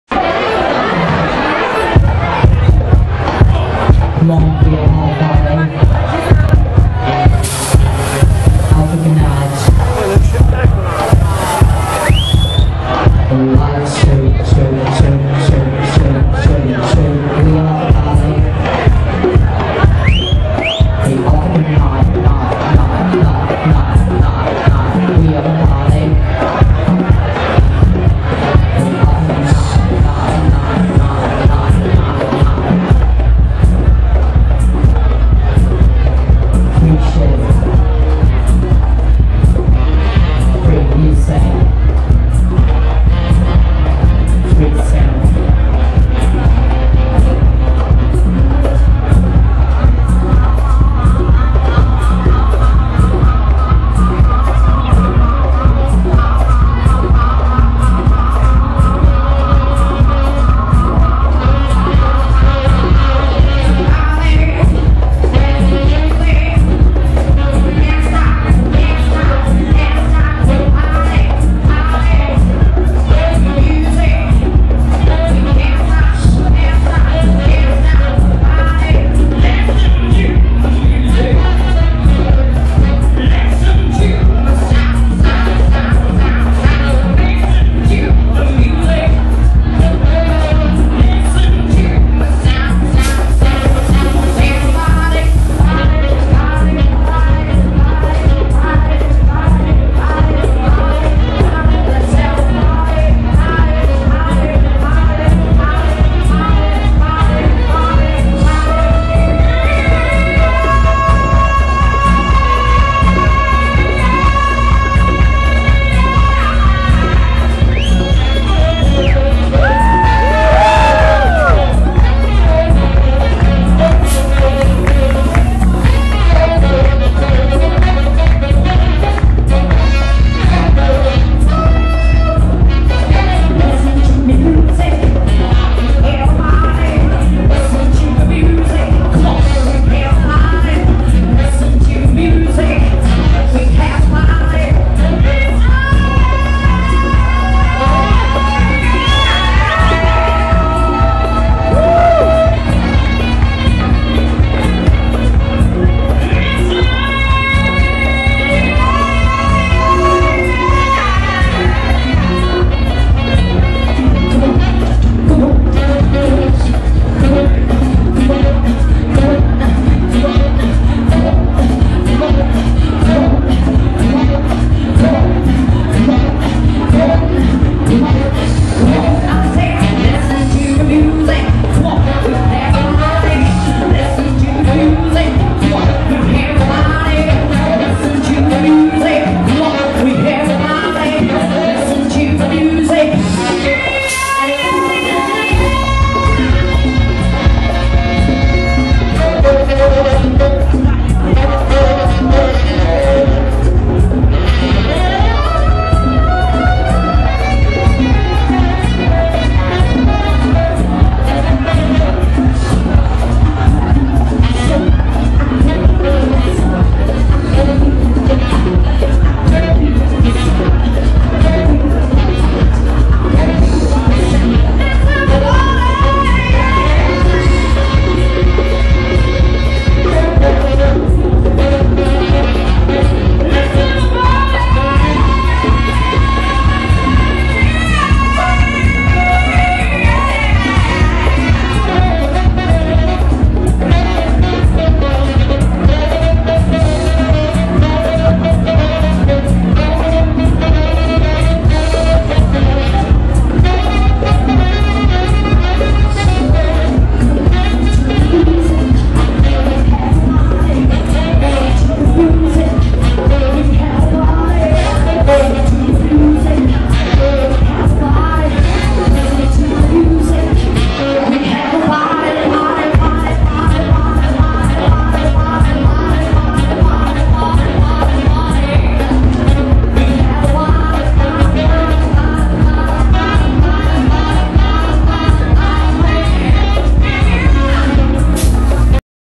Art Cafè Live Afro Party 2010 part 2
Art Cafè Live Roma Afro Party 2010
From Rome Valentina Ferrari Voice,from Boston Eric Daniel Sax,from Brooklyn Simba Percussioni.